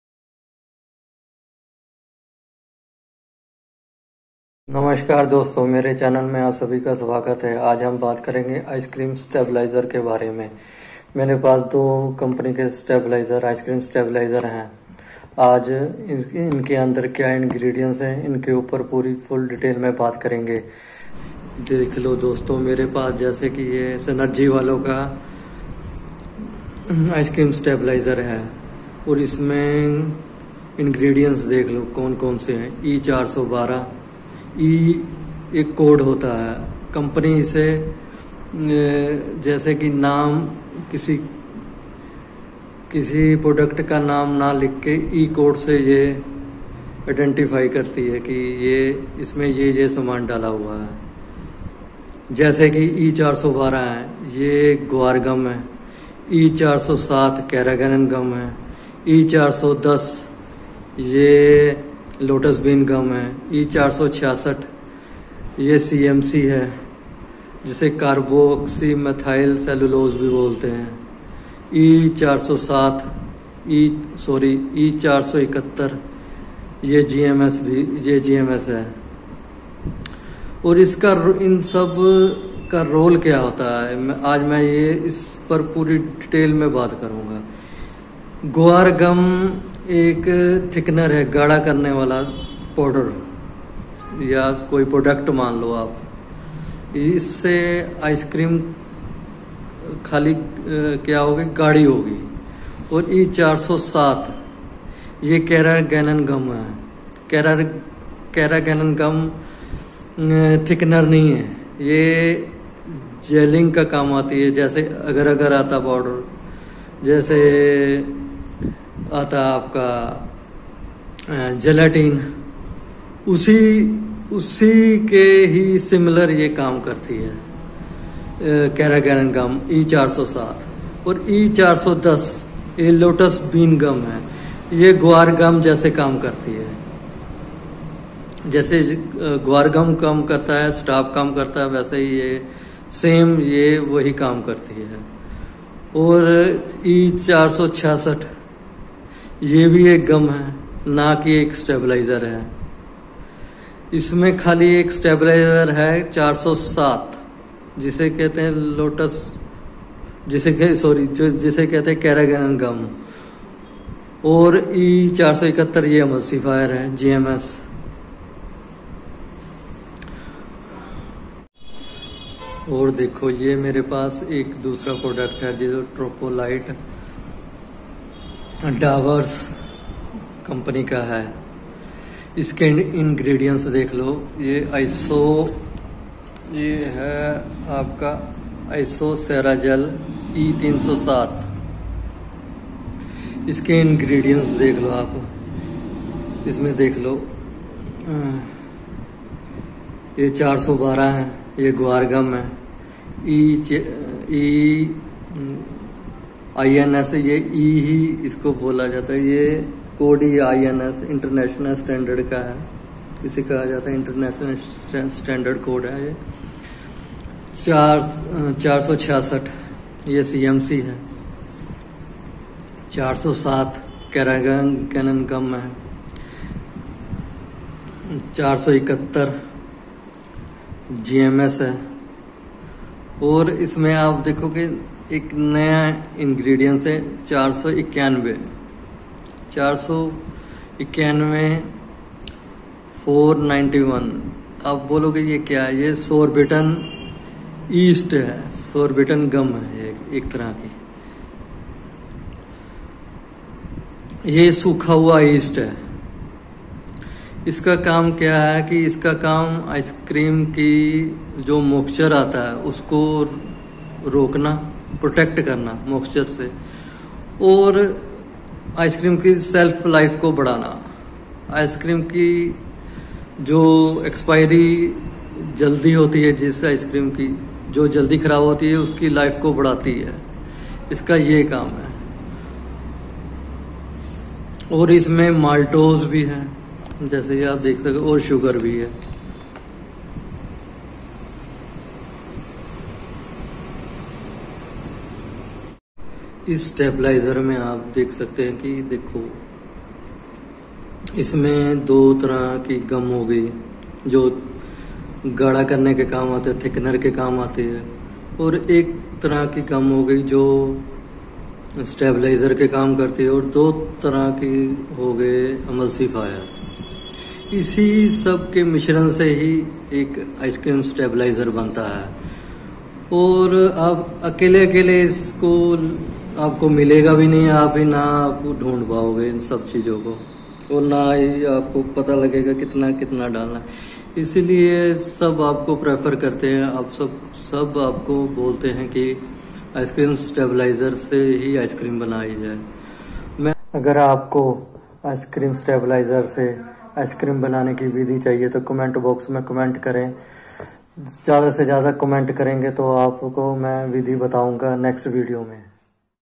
Ice Cream Emulsifier and Stabilizer work explain in hindi/emulsifier & stabilizer role in ice cream
ice cream Emulsifier and STABILIZER FULL DETAILS IN HINDI |ice cream stabilizer|